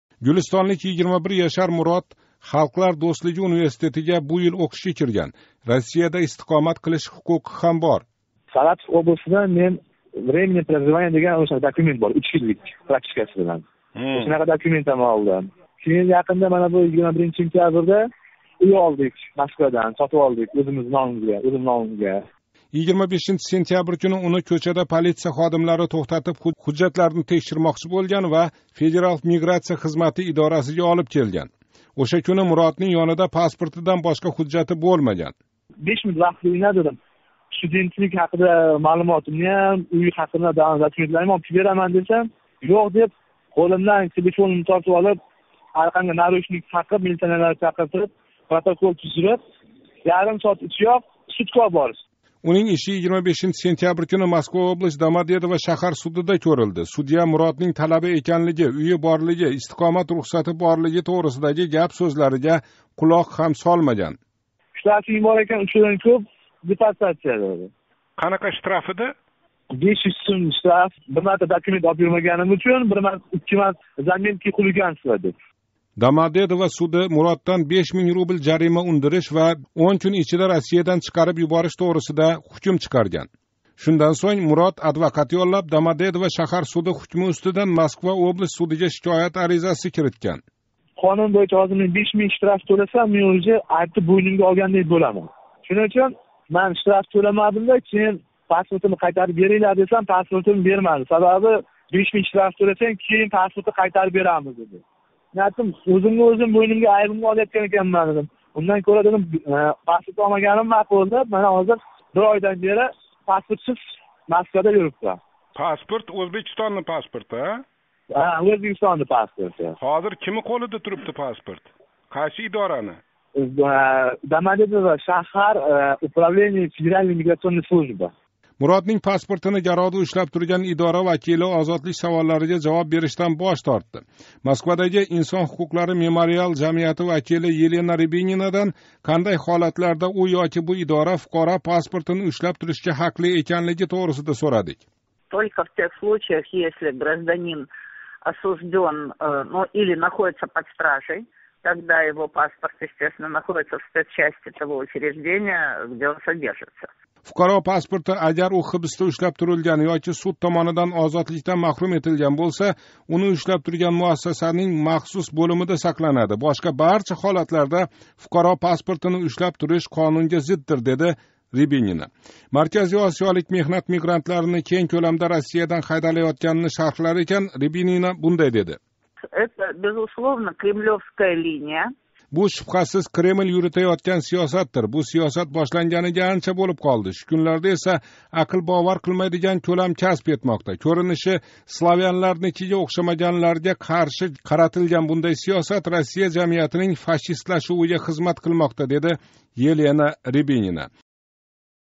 Москвада ўз уйи бор ўзбек талабаси депорт қилинишини кутмоқда
Кейинги пайтларда Россия жамиятининг фашистлашуви кузатилмоқда. Бу Кремль олиб бораётган сиёсат оқибатидир, деди Озодлик билан суҳбатда инсон ҳуқуқлари "Мемориал" жамияти вакили.

Елена Рябинина билан Москвадаги Халқлар дўстлиги университети 1 курс талабаси, сирдарёлик Муроднинг Озодликка шикоятидан сўнг суҳбатлашдик.

Мурод ҳам минглаб ватандошлари каби яқин орада Россиядан чиқариб юборилиши мумкин.

Гулистонлик 21 яшар Мурод (фамилияси айтилмаслигини истади) Халқлар дўстлиги университетига бу йил ўқишга кирган. Россияда истиқомат қилиш ҳуқуқи ҳам бор.

- Саратов областидан вақтинча истиқомат қилишга 3 йиллик рухсатнома олганман, сентябрда эса Москвадан менинг номимга уй сотиб олганмиз, Москвада ҳам вақтинчалик қайддан ўтганман, деди Мурод.

25 сентябрь куни уни кўчада полиция ходимлари тўхтатиб, ҳужжатларини текширган ва Федерал миграция хизмати (ФМС) идорасига олиб келган.
Ўша куни Муроднинг ёнида паспортидан бошқа ҳужжати бўлмаган.

- 5 минут вақт берсангизлар уйим борлигини, талаба эканлигимни тасдиқловчи ҳужжатларимни олиб келиб кўрсатаман, десам, йўқ, деб телефонимни тортиб олишди, қўлларимга кишан солиб ярим соатнинг ичида судга олиб боришди, дейди Мурод.

Унинг иши 25 сентябрь куни Москва область, Домодедово шаҳар судида кўрилди. Судья Муроднинг  талаба эканлиги, уйи борлиги, истиқомат рухсати борлиги тўғрисидаги гап-сўзларига қулоқ ҳам солмаган.